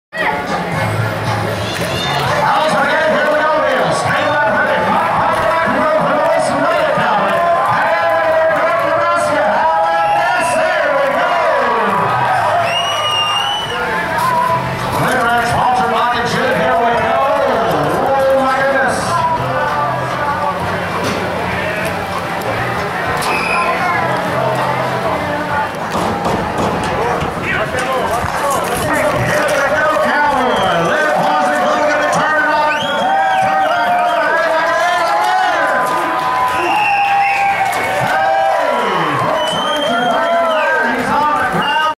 Bull Riding Friday
Bull Riding at Friday night's performance of the 2012 Sheridan County Fair and Rodeo